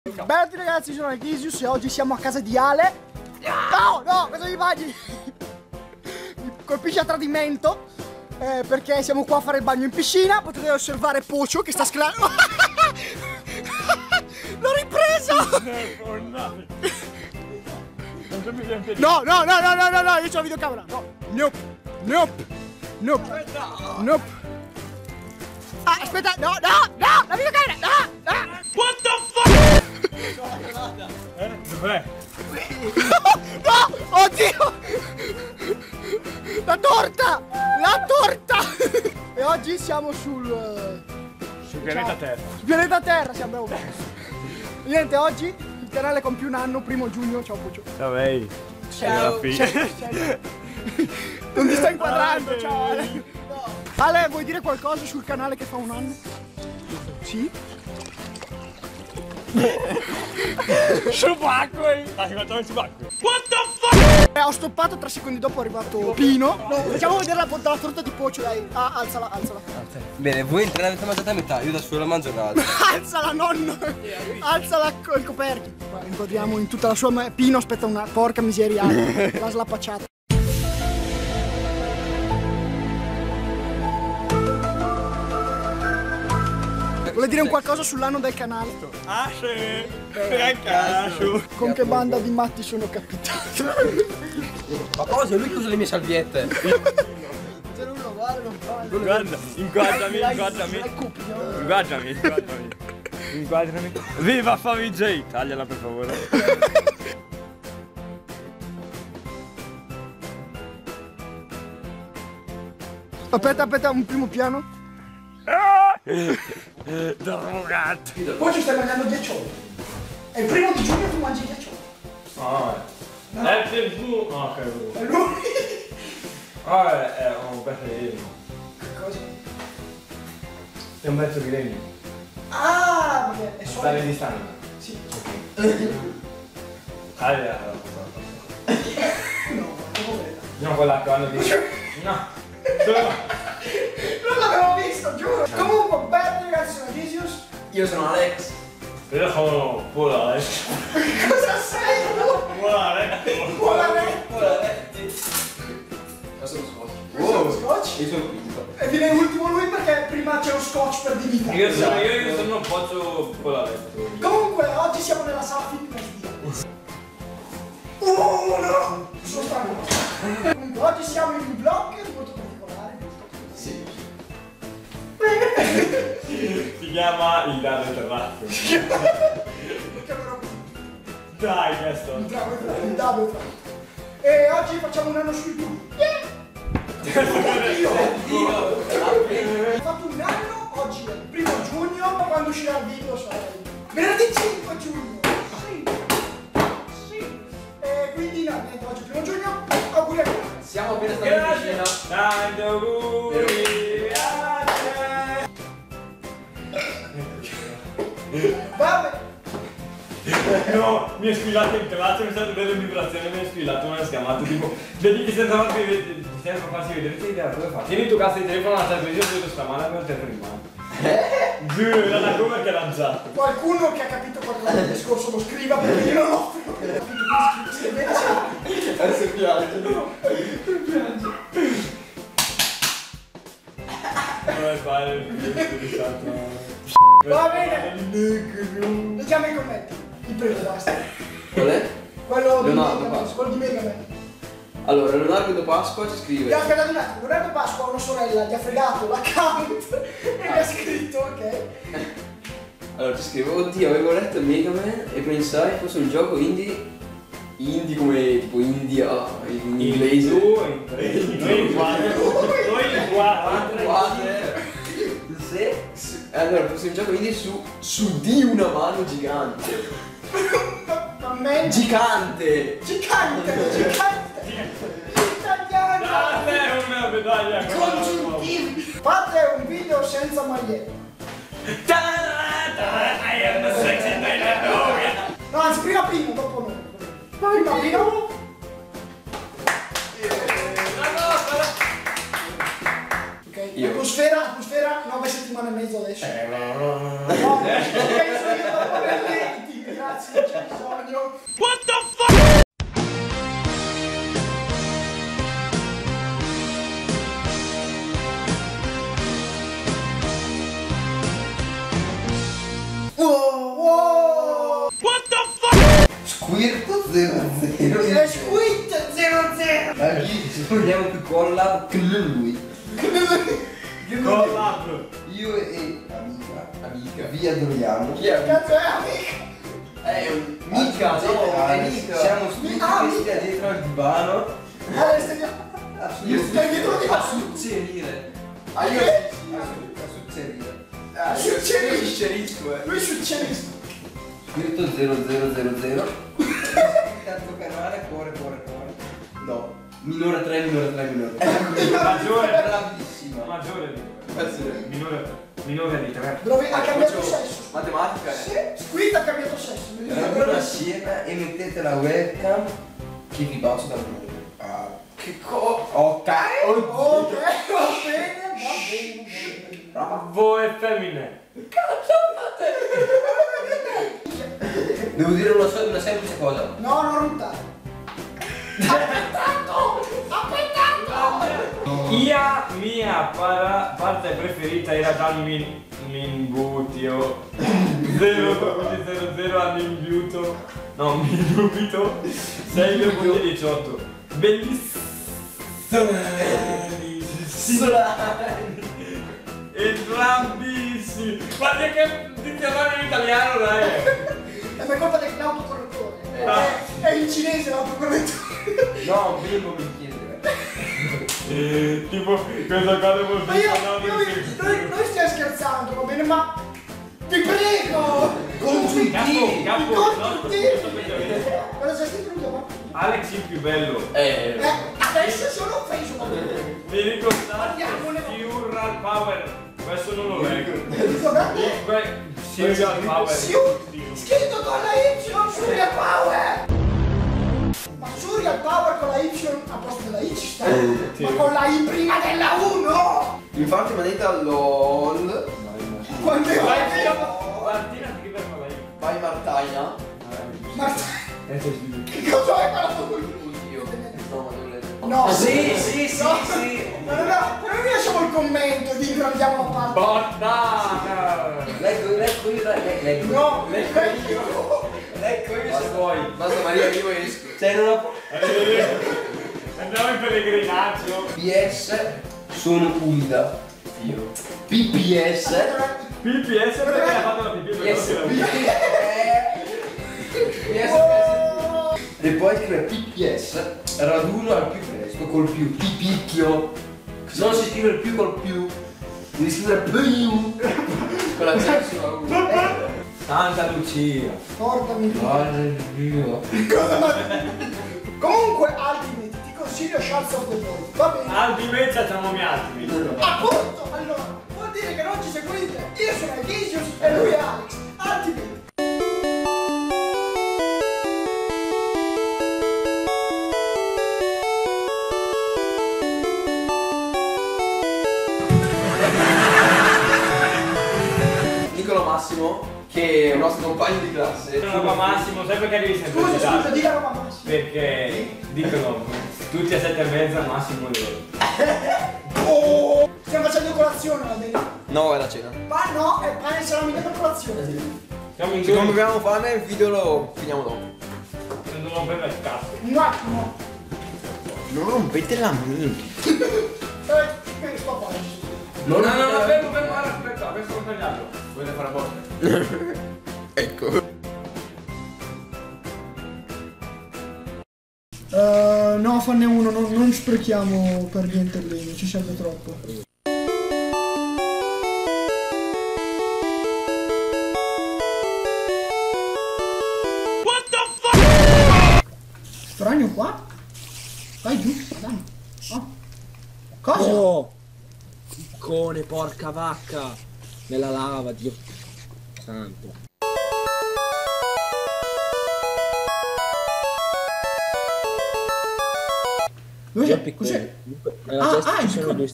1 Anno su Youtube - Il Dado è Tratto [With Pino & Pocho]
Sembra incredibile sia già passato un anno... Eppure, come si suol dire, il tempo vola quando ci si diverte. Abbiamo quindi deciso di fare un video su questa giornata fantastica a casa di Alex insieme a Pino e Pocho. Nel video abbiamo scherzato, riso e ci siamo divertiti. Ci scusiamo se abbiamo preso in giro qualcuno e vi diciamo che il nostro intento non è ridere di voi ma con voi. Vi diciamo anche che da questo momento in poi i video che usciranno a settimana torneranno ad essere 3 quando possibile. Vi auguriamo anche BUONE VACANZE ^_^